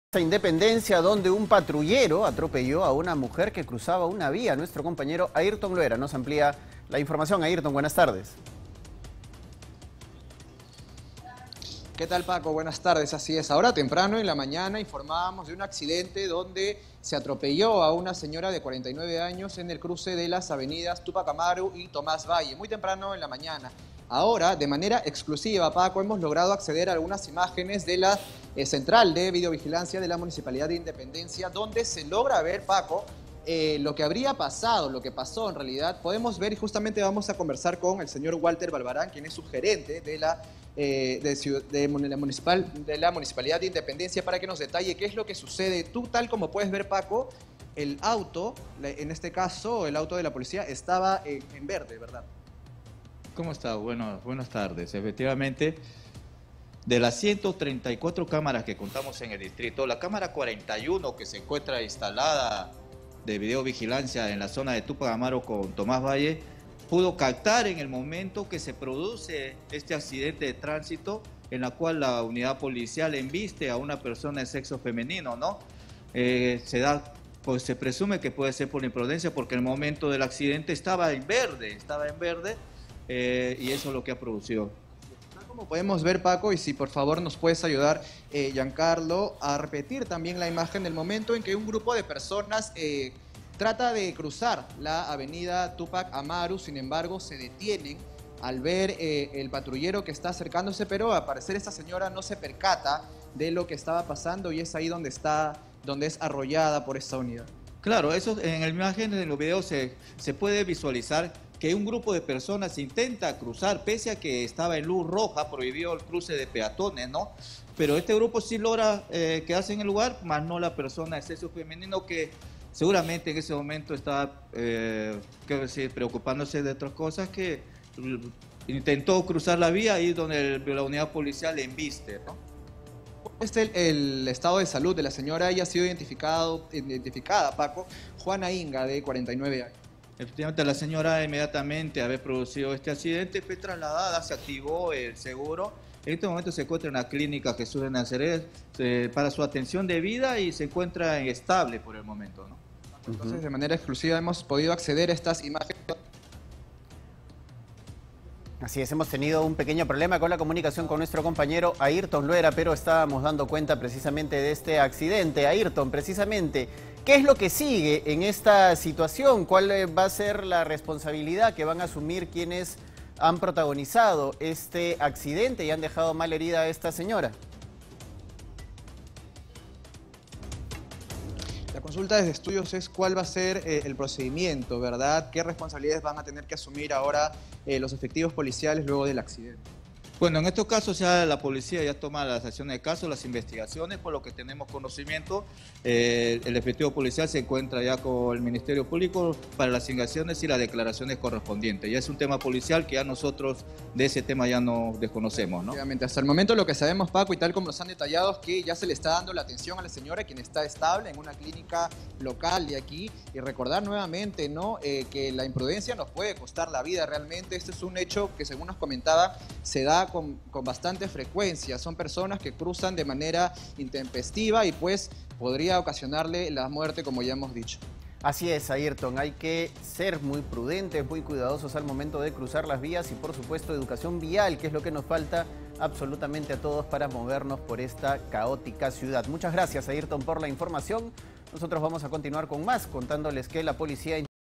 Cámaras de seguridad muestran que mujer cruzó en rojo cuando fue atropellada por patrullero
Durante la mañana del día de hoy, un patrullero atropelló a una señora y la dejó herida en el cruce de la avenida Tomás Valle y Túpac Amaru, en el distrito de Independencia. Al principio se culpó al agente policial, sin embargo, cámaras de seguridad comprobarían que la imprudencia de la agraviada también contribuyó al accidente.

Imágenes registran que la patrulla hizo uso de la vía exclusiva del Metropolitano, cuando un grupo de personas cruzó intempestivamente, a pesar de que la luz estaba en rojo para peatones. No obstante, la mayoría de la gente se detuvo al percatarse del vehículo policial, excepto la mujer de 49 años que fue arrollada.

La víctima, identificada como Juana Inga, fue llevada a la clínica Jesús de Nazaret donde se encuentra estable de salud.

Por otro lado, los efectivos policiales responsables del accidente se han dirigido al Ministerio Público para declarar respecto al caso.

Cabe resaltar, se le señaló al policía que circuló por una vía prohibida, él se justificó alegando estar trasladando a un detenido, cuando no es una situación de emergencia comprobada, tal como figura en la Ley 27 200.